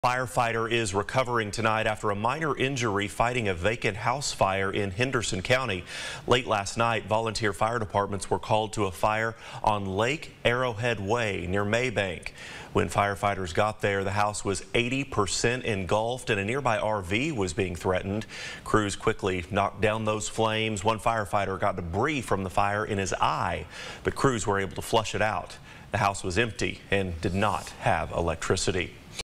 Payne Springs crews respond to mobile home fire
Crews responded to reports of a structure fire on Lake Arrowhead that destroyed a home.